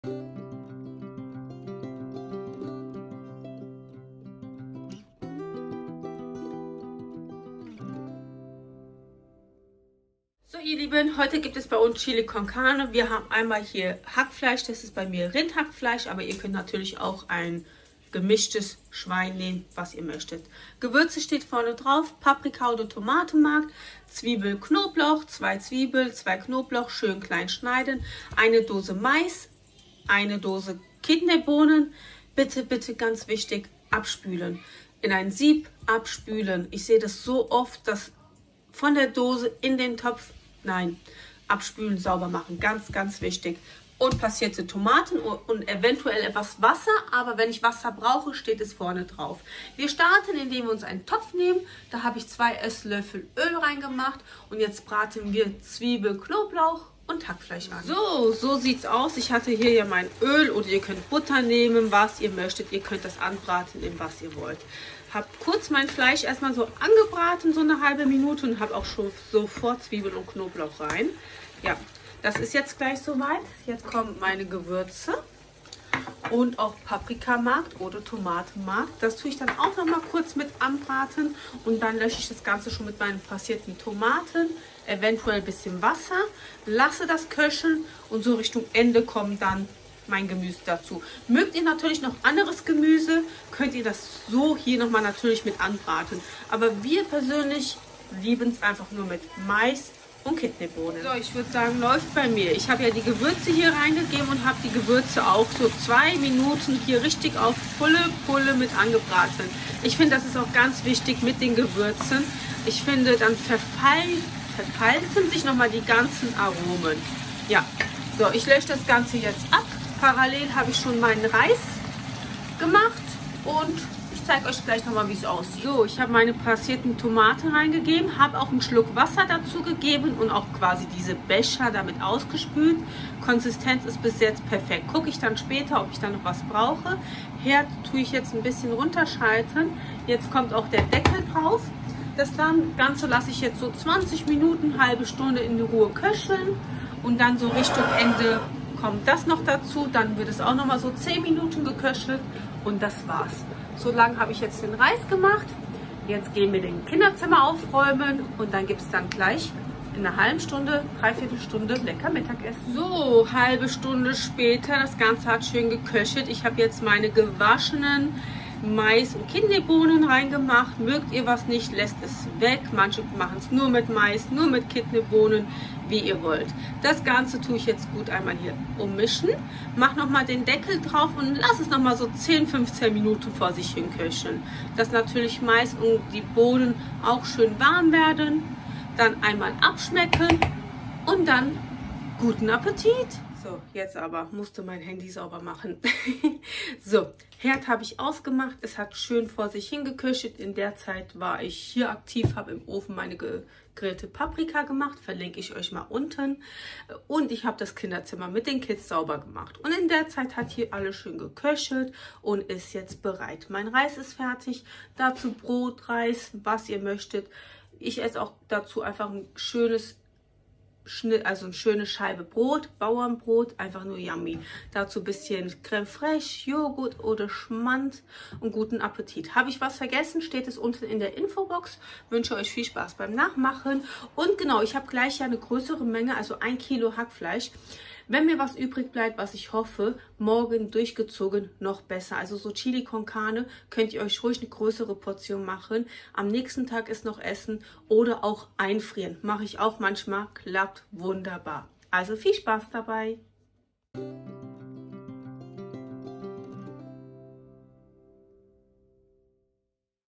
Chili con carne / einfache Chili con carne / kochen / lecker einfach kochen
Heute zeige ich euch einmal Chili Con Carne.
Es ist lecker und geht eigentlich immer.
 Wünsche euch viel Spaß beim nachmachen.
Habe eigentlich alles im Video gesagt.
Mengen Angaben und Zutatenliste findet ihr Anfang im Video gezeigt.